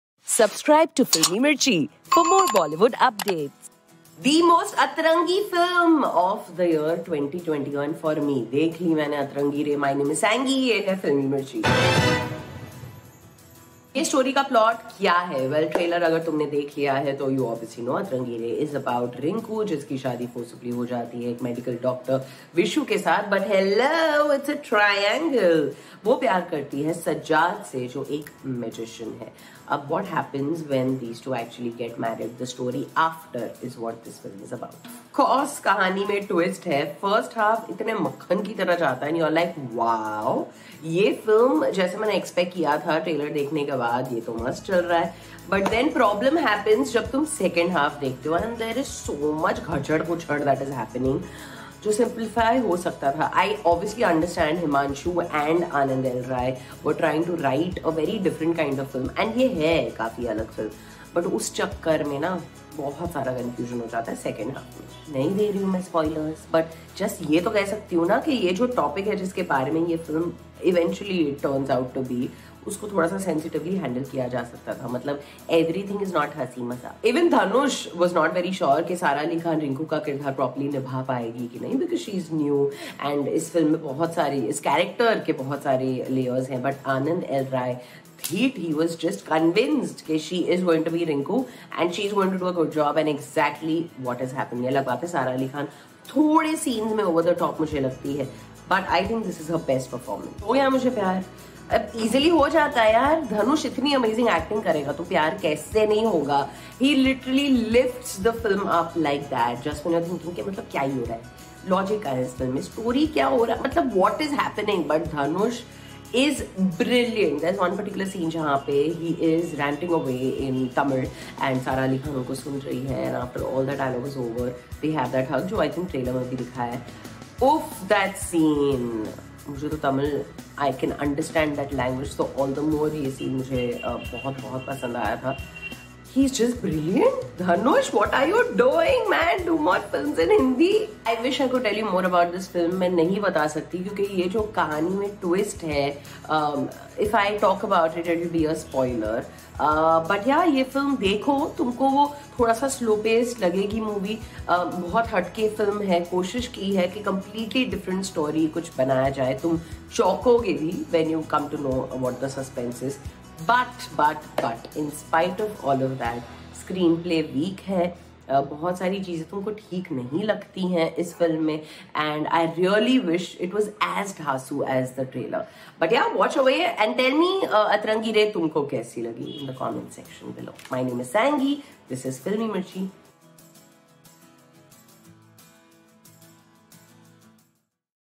Atrangi Re Review | Akshay Kumar | Dhanush | Sara Ali Khan
Did Atrangi Re match RJ Sangy's Atrangi Meter? Watch the full review now!